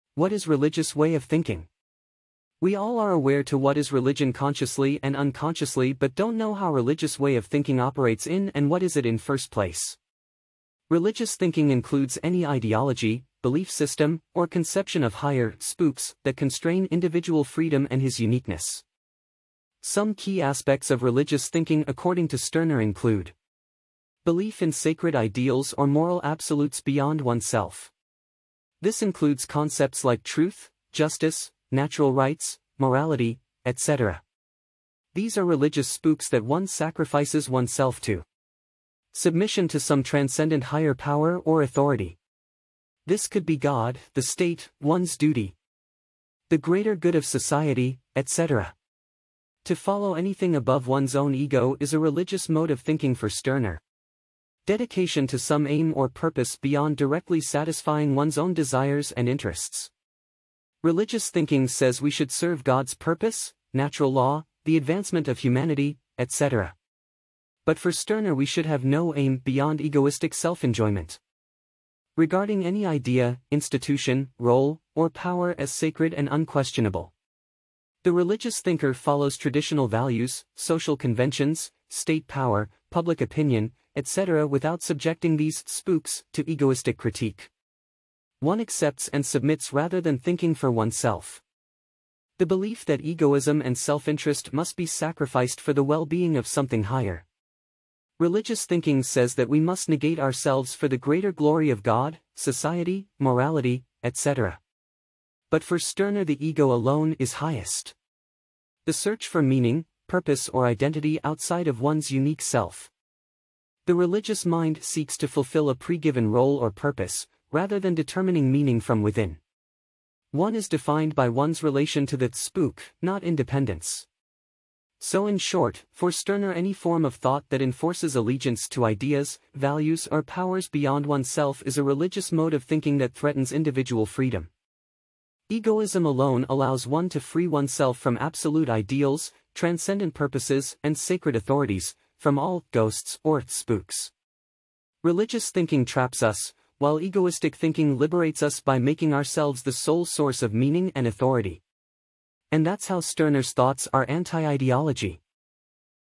Max Stirner - What is "Religious way of Thinking."
What is religious way of thinking. We all are aware to what is religion consciously and unconsciously but don't know how religious way of thinking operates in and what is it in first place.

Religious thinking includes any ideology, belief system, or conception of higher "spooks" that constrain individual freedom and his uniqueness. Some key aspects of religious thinking according to Stirner include:

Belief in sacred ideals or moral absolutes beyond oneself. This includes concepts like Truth, Justice, Natural Rights, Morality, etc. These are religious spooks that one sacrifices oneself to.

Submission to some transcendent higher power or authority. This could be God, the State, one's duty, the greater good of society, etc. To follow anything above one's own ego is a religious mode of thinking for Stirner.

Dedication to some aim or purpose beyond directly satisfying one's own desires and interests. Religious thinking says we should serve God's purpose, natural law, the advancement of humanity, etc. But for Stirner we should have no aim beyond egoistic self-enjoyment.

Regarding any idea, institution, role, or power as sacred and unquestionable. The religious thinker follows traditional values, social conventions, state power, public opinion, etc. without subjecting these "spooks" to egoistic critique. One accepts and submits rather than thinking for oneself.

The belief that egoism and self-interest must be sacrificed for the well-being of something higher. Religious thinking says that we must negate ourselves for the greater glory of God, society, morality, etc. But for Stirner the ego alone is highest.

The search for meaning, purpose or identity outside of one's unique self. The religious mind seeks to fulfill a pre-given role or purpose, rather than determining meaning from within. One is defined by one's relation to the "spook", not independence.

So in short, for Stirner any form of thought that enforces allegiance to ideas, values or powers beyond oneself is a religious mode of thinking that threatens individual freedom. Egoism alone allows one to free oneself from absolute ideals, transcendent purposes and sacred authorities - from all "ghosts" or "spooks". Religious thinking traps us, while egoistic thinking liberates us by making ourselves the sole source of meaning and authority. And that's how stirner's thoughts are anti-ideology.